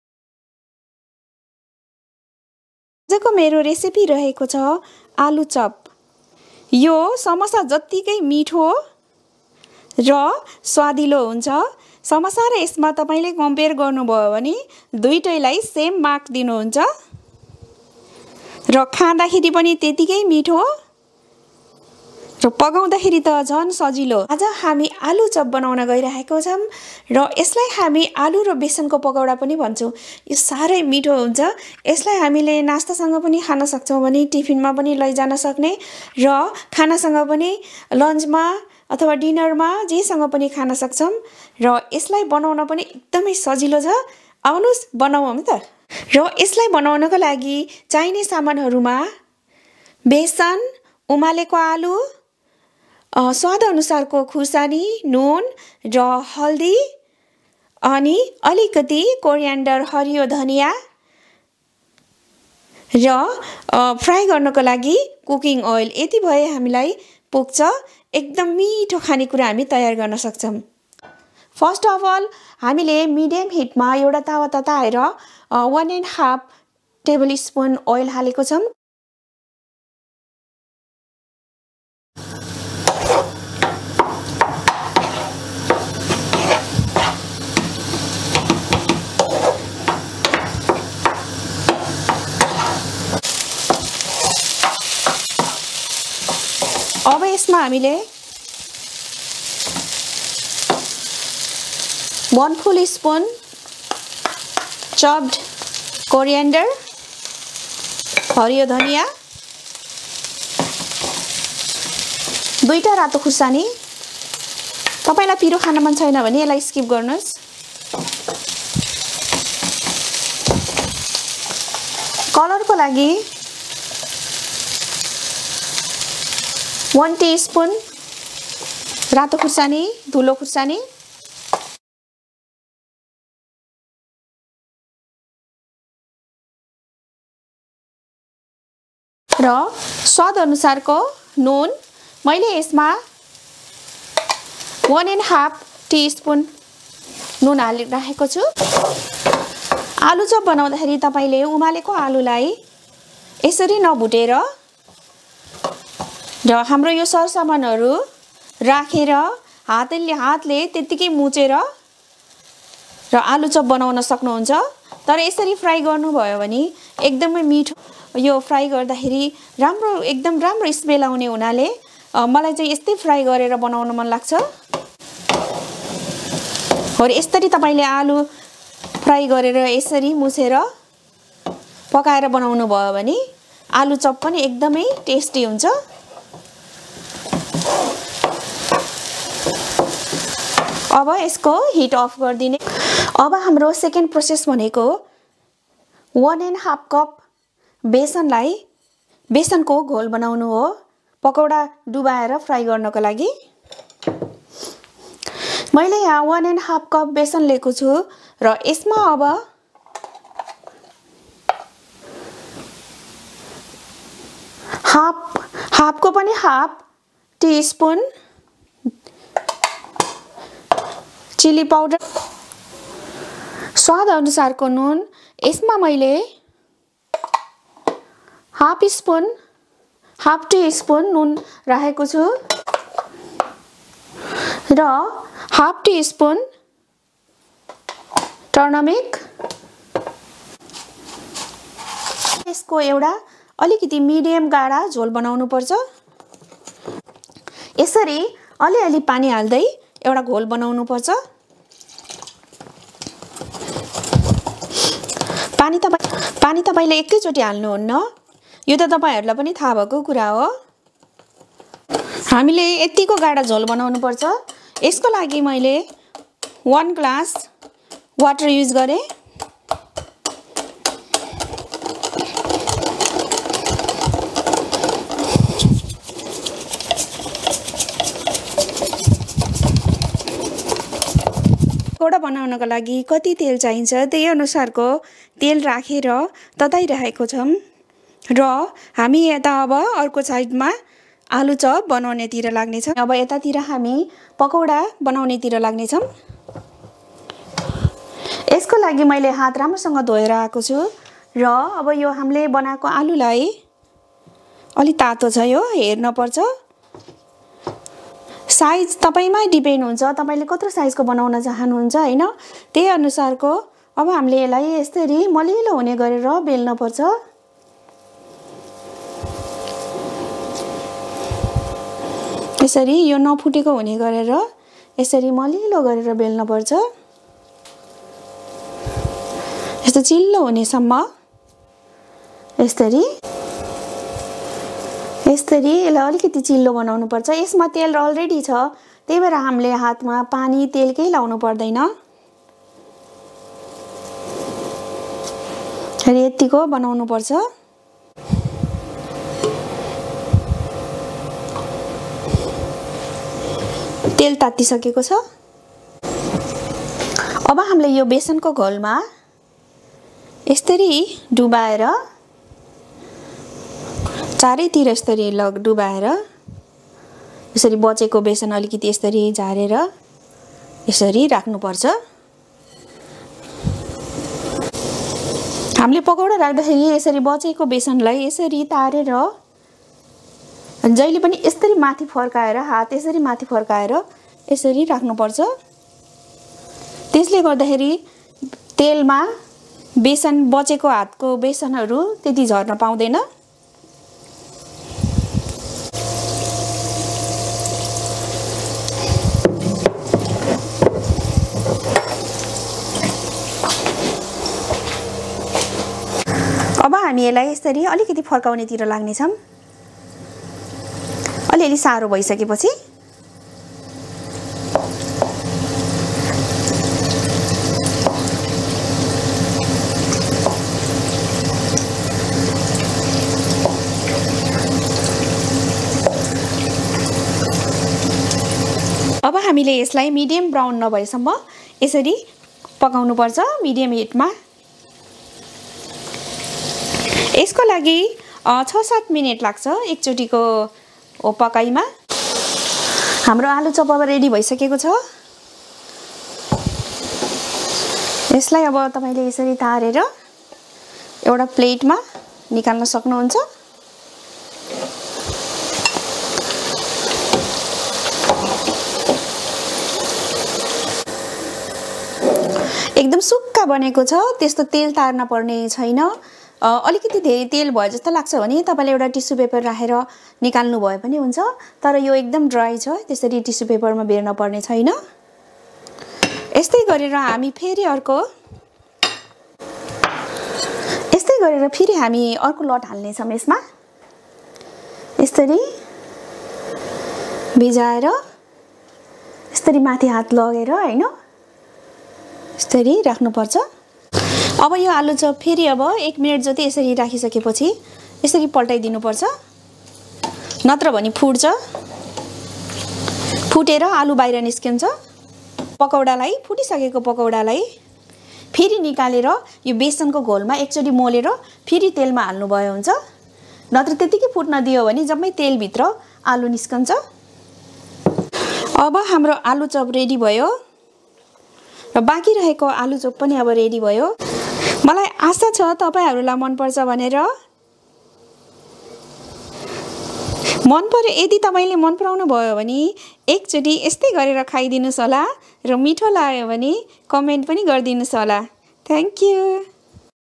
Aloo Chop|| Spicy Potato Chop || Spicy Aloo Pakoras || टवाक्क परेको स्पाइसि अ।लु चप
Beson ( Gram Flour )= 1 1/2 cup
cooking oil = 1 1/2 spoon
Fresh red Chilli  = 2
Chilli Powder = 1 tsp
Fresh Coriander = 1 spoon
(How to Make Aloo Chop || Nepali-Indian Street Food||Muna and Family Vlogs || Potato Recipes|| Nepali Speaking || Nepali in UK)